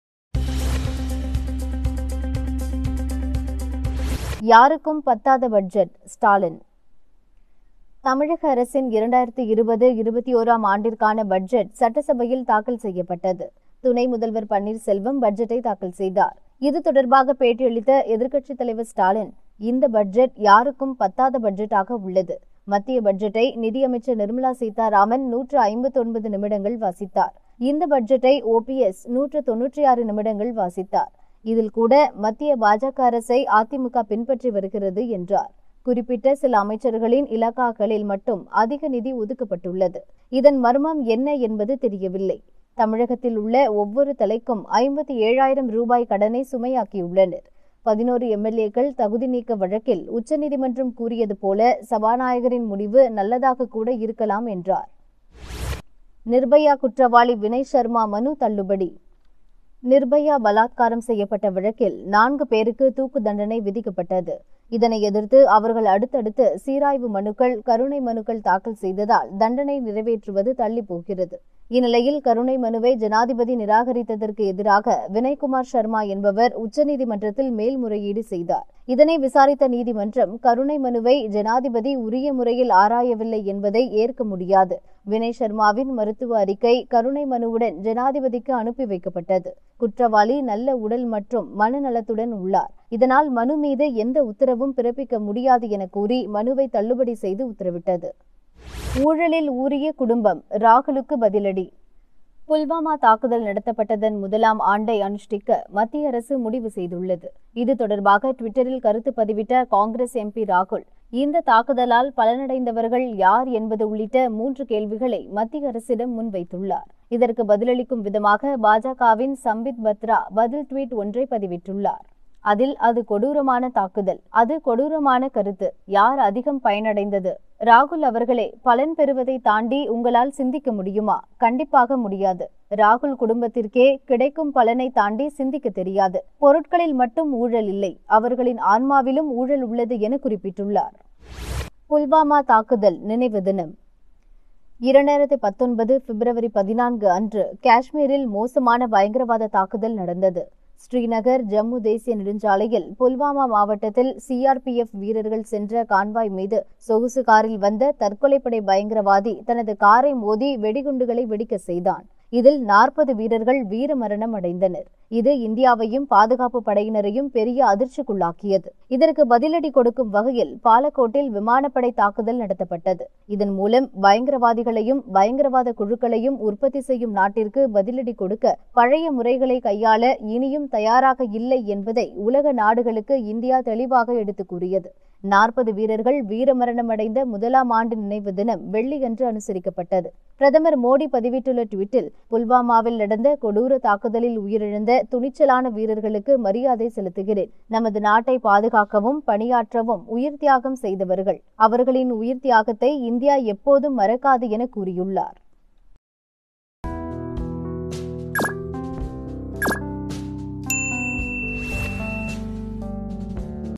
செய்திச்சுருக்கம் | Seithi Surukkam 14-02-2020 | மாலை 4 மணி | Dinamalar
01.யாருக்கும் பத்தாத பட்ஜெட்: ஸ்டாலின்
02.புல்வாமா தாக்குதல் நினைவு தினம்
03.ஊழலில் ஊறிய குடும்பம்: ராகுலுக்கு பதிலடி